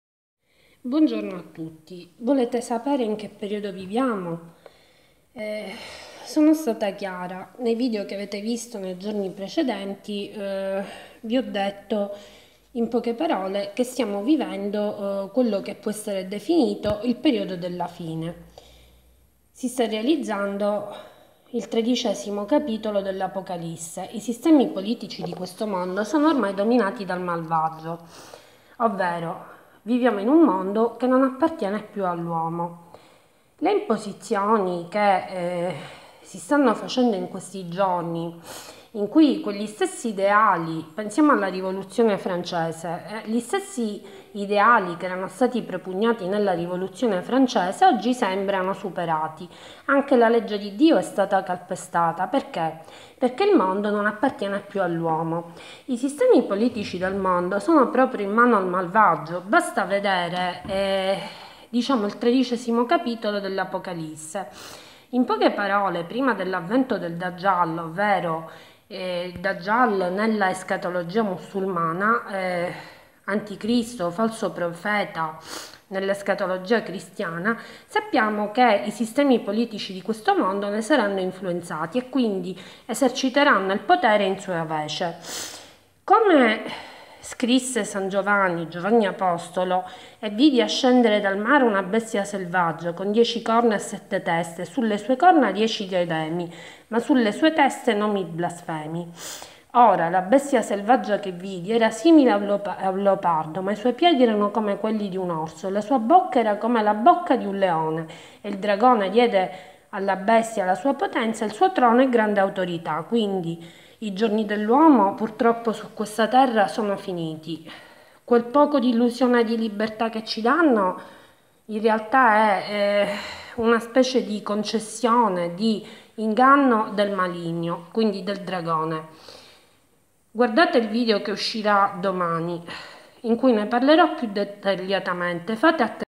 I sistemi politici di questo mondo sono dominati dal dragone che e' Satana il principe del mondo
Fratelli prestate attenzione perche' questo mondo non appartiene piu' all' uomo si sta realizzando cio' che l' apostolo Giovanni scrisse nel 13 capitolo dell' Apocalisse versetti 1-3 prima dell' avvento del Dajjial la cui fitna e' gia' sparsa sulla terra. Fratelli guardatevi quindi  ed uscite dal mondo che ormai e' in mano al dragone ed a governi corrotti. Guardatevi dalla fitna del Dajjial che presto si manifestera' al mondo. Pregate piuttosto perche' il messia verra' con i suoi  angeli nella sua gloria al termine di questo sistema di cose per giudicare il malvagio. Dal Chrislam